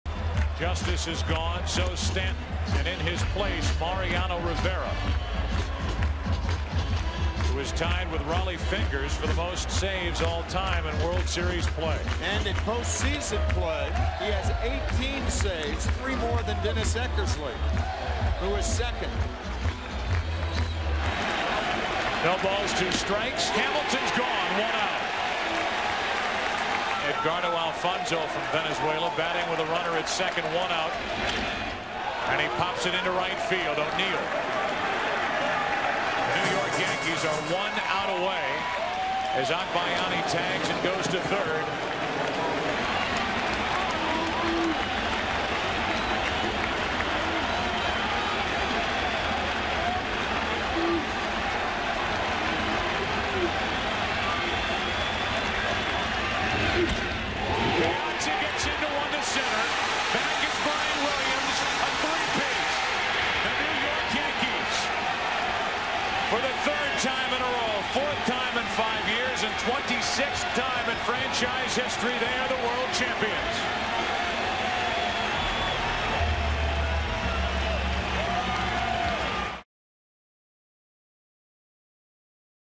2000 WS Gm5: Three-peat! Mo seals World Series
10/26/00: Mariano Rivera throws a scoreless ninth to secure the save and a Yankees three-peat in the 2000 World Series against the Mets.